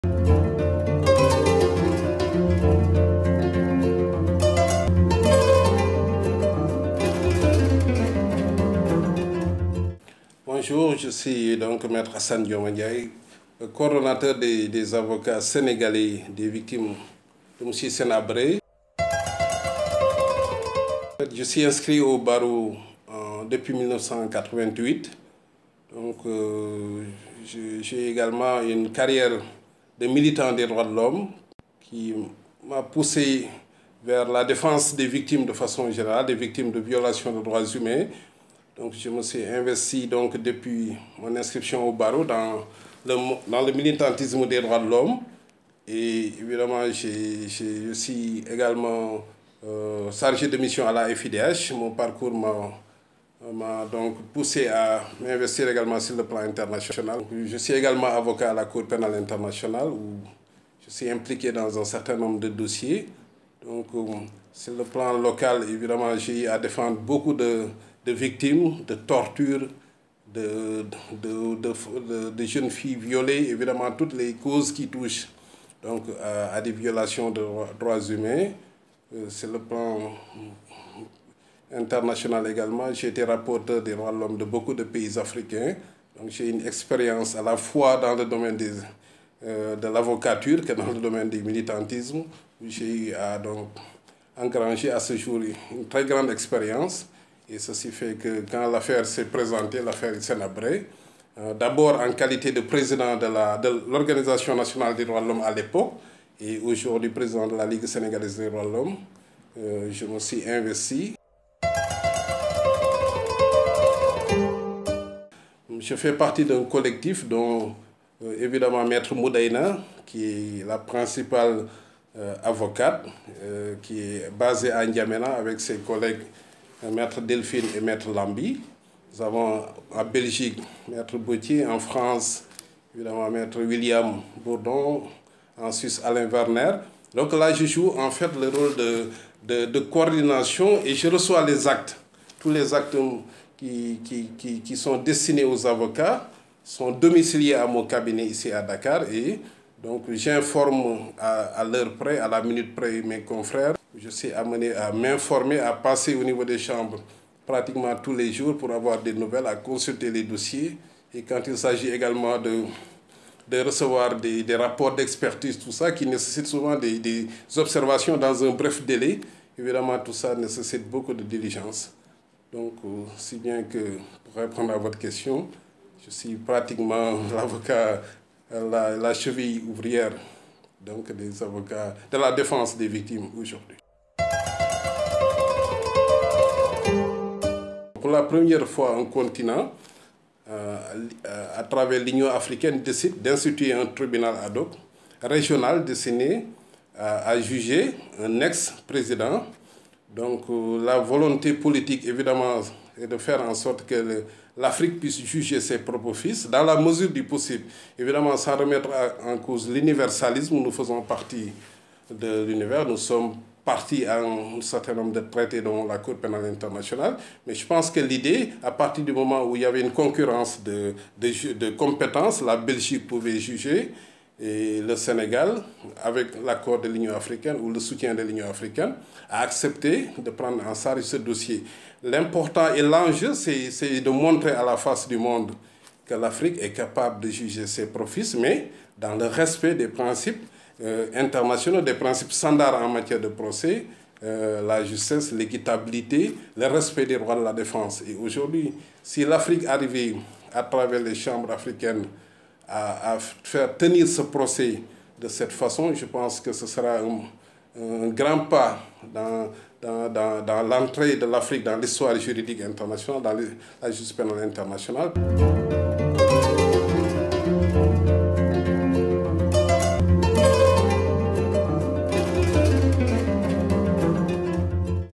Interview Me Assane Dioma Ndiaye
Me Assane Dioma Ndiaye est Avocat conseil sénégalais de l’Association des victimes des crimes du régime Hissein Habré (AVCRHH)